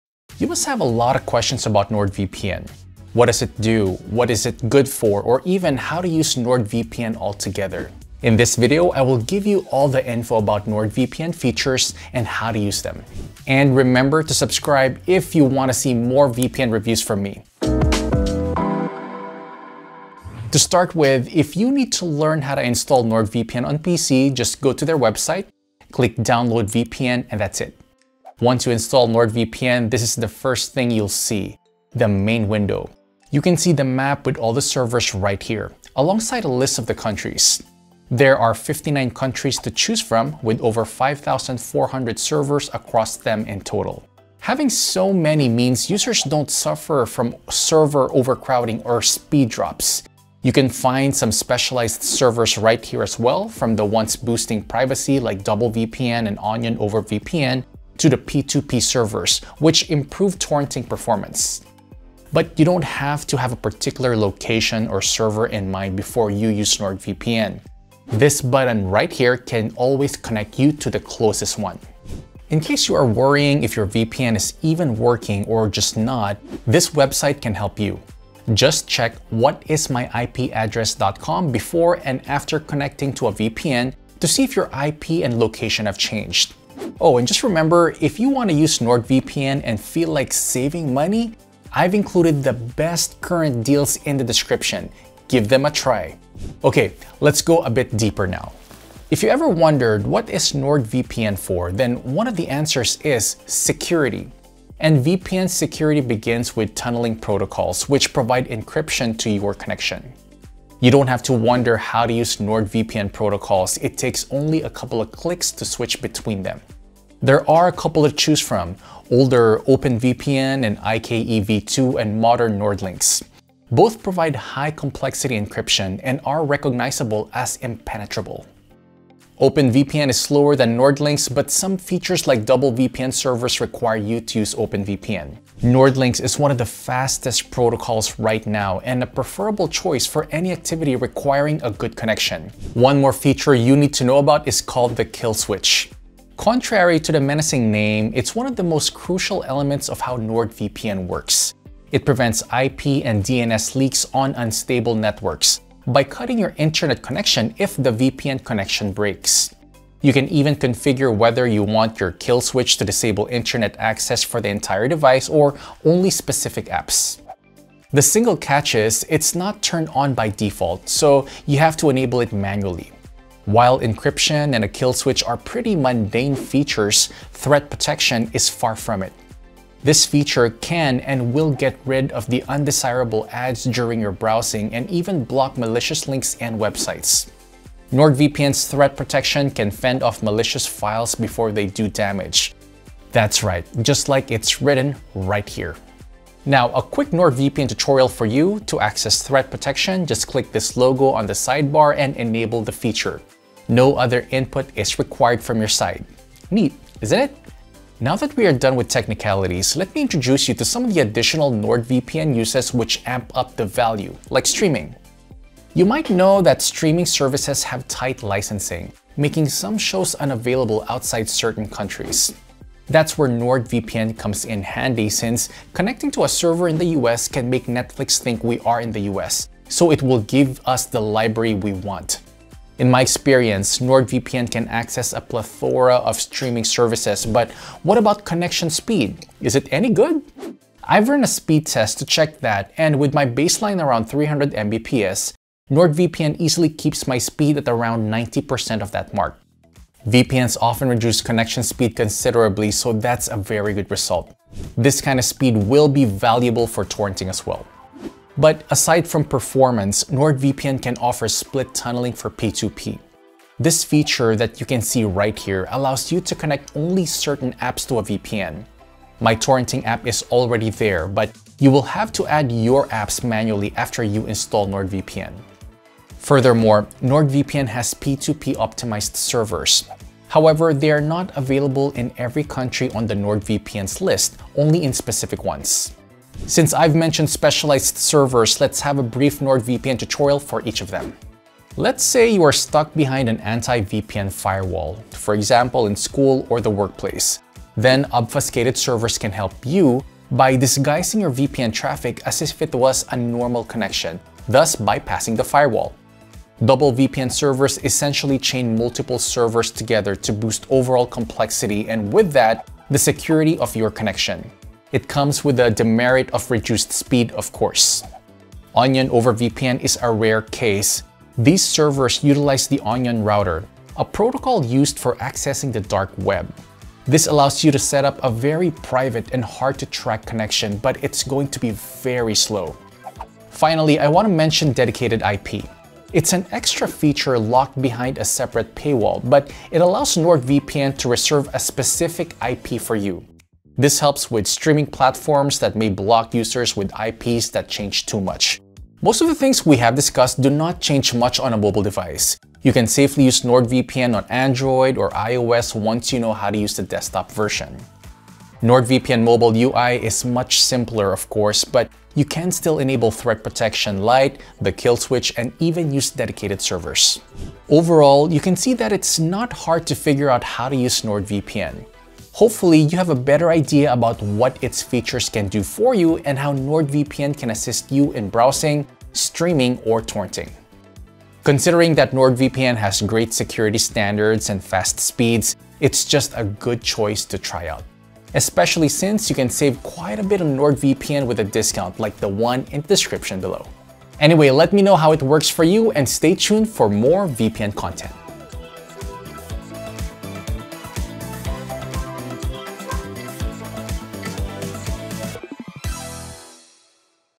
How To Use NordVPN | Full NordVPN Explanation + TUTORIAL
💥👇 Check NordVPN with the best discounts available right now! 👇💥

You must have a lot of questions about NordVPN. What does it do, what is it good for, how to use NordVPN at all? In this video, I will give you all the info you need to bring out NordVPN’s full potential while answering the most popular questions about NordVPN features.

To start with, if you need to learn how to install NordVPN on PC, just go to their website, click “Download VPN” at the top of the page and get to downloading.

Once you install NordVPN, the first thing you see is the main window. You will be able to see the map with all the servers right here, alongside the list of the countries. There are 59 countries to choose from, with over 5,200 servers across them in total.

Having so many is important since that allows NordVPN to host more people without worrying about server overcrowding and speeds going down.

You can find some specialized servers right here as well, from the ones boosting privacy like  Double VPN and Onion over VPN to the P2P servers, which improve performance.

But you don’t have to have a particular location or server in mind before you use NordVPN. This button right here can always connect you to the closest one.

In case you are worrying if your VPN is even working or not, there is a website that can help you. Just check whatismyipaddress.com before and after connecting to a VPN to see if your IP and location have changed.

=== Tunneling protocols ===

If you ever wondered what is NordVPN for, then one of the answers is “Security”. And VPN’s security begins with tunneling protocols, which provide encryption to your connection.

You don’t have to wonder how to use NordVPN protocols, it takes only a couple of clicks to switch between them. There are a couple to choose from: older OpenVPN and IKEv2 and modern NordLynx. Both provide high-complexity encryption and are recognized as impenetrable.

OpenVPN is slower than NordLynx, but some features like DoubleVPN servers require you to use OpenVPN.

NordLynx is one of the fastest protocols right now and a preferable choice for any activity requiring a good connection.

=== Kill switch ===

One more feature you need to know about is called the Kill switch. Contrary to the menacing name, it’s one of the most crucial elements of how NordVPN works. It prevents IP and DNS leaks on unstable networks, by cutting your internet connection if VPN connection breaks. You can even configure whether you want your Kill switch to disable internet access for the entire device or only specific apps. The single catch is - it’s not turned on by default, so you have to enable it manually.

=== Threat protection ===

While encryption and Kill switch are pretty mundane features, Threat protection is far from it. This feature can and will get rid of the undesired ads during your browsing and even block malicious links and websites. NordVPN’s Threat protection can even fend off malicious files before they do damage. That’s right, just like it’s written right here! Here’s a quick NordVPN tutorial for you: to access Threat protection, just click this logo on the sidebar and enable the feature. No other input is required from your side. Neat, isn’t it?

\\TIMESTAMPS//
0:00 Intro
0:20 How to install NordVPN?
0:50 How to connect to NordVPN?
1:36 What is NordVPN for?
2:22 How to use NordVPN Kill Switch?
2:27 How to use NordVPN Threat Protection?
3:34 Is NordVPN fast enough for streaming?
4:15 How to use NordVPN for torrenting?
4:46 What other features does NordVPN offer?
5:48 How to use NordVPN on mobile?
6:07 Conclusion